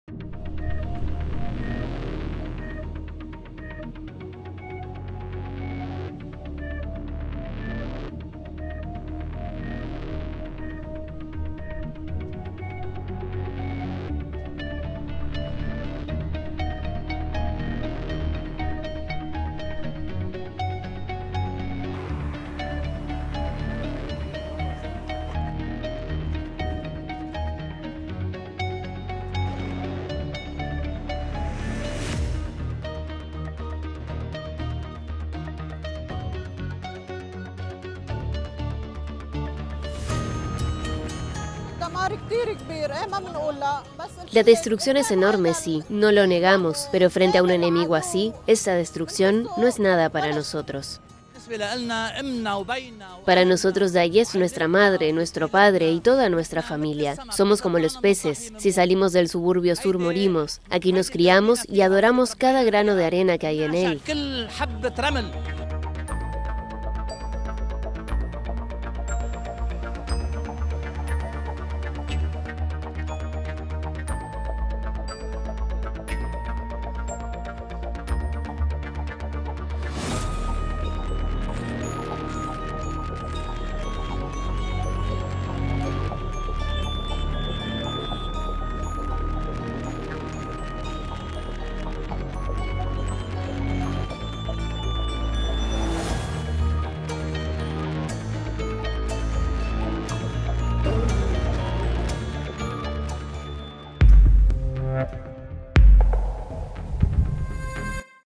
Con el alto al fuego entre Israel y Hezbolá en Líbano miles regresan a casa
La tregua, interrumpe un conflicto que ha dejado miles de muertos y 900.000 desplazados en Líbano, además de decenas de miles de evacuados en el norte de Israel.